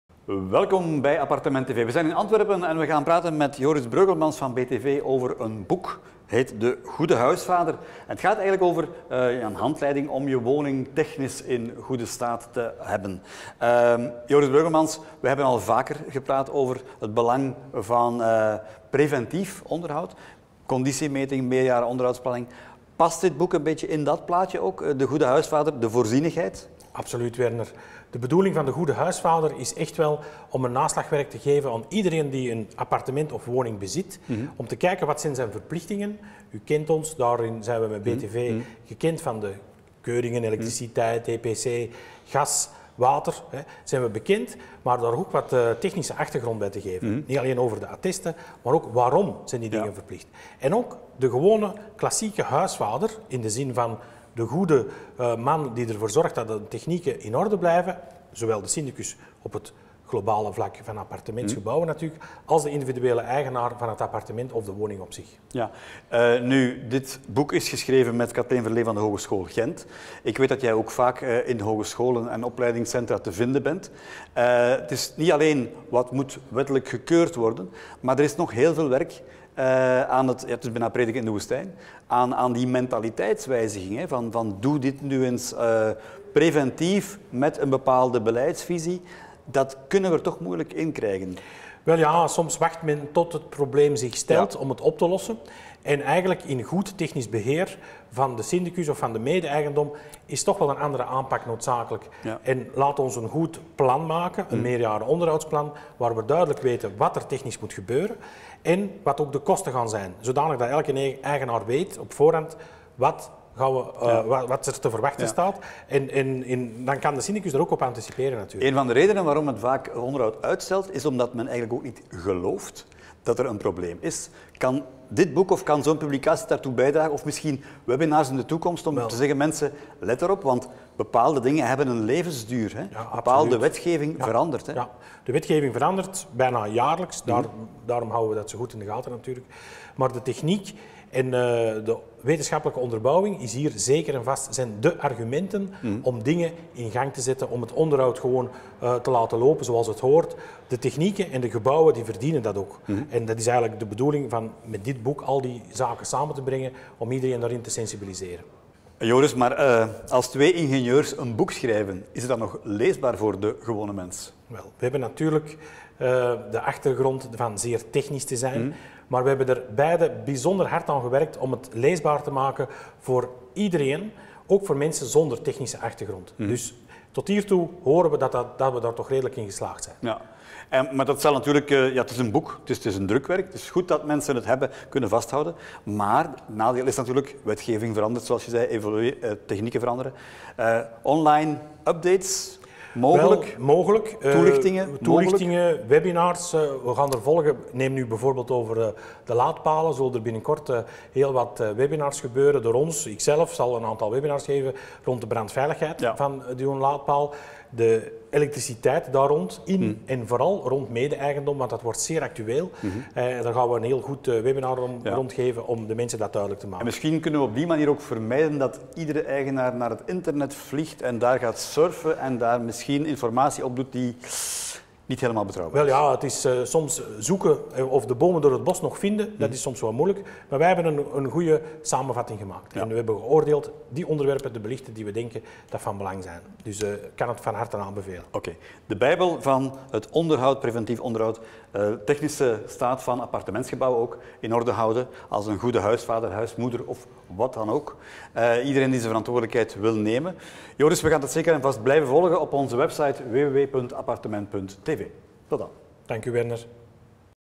BOEK De Goede HUISvader - co-auteur - BTV - Joris Breugelmans
Werner Couck sprak met co-auteur Joris Breugelmans over het boek "De Goede HUISvader" dat een leidraad moet zijn om je woning of appartement in goede conditie te houden.